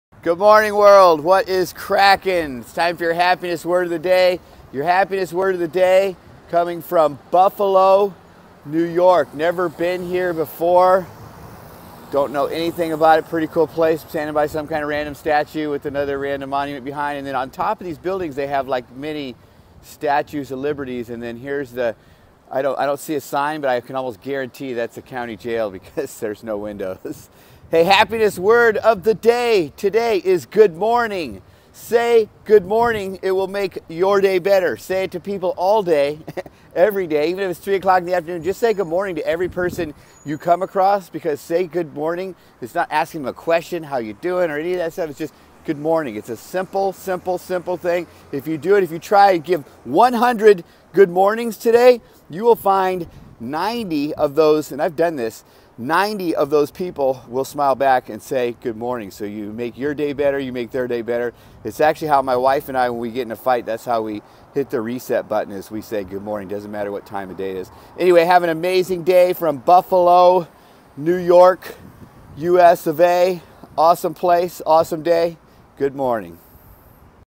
Word of the day - Good Morning - Resetting Patterns
Simple gestures are often overlooked and taken for granted. Instead of fitting in with everyone else, stand out by changing patterns. Find out how two simple words can bring about transformation.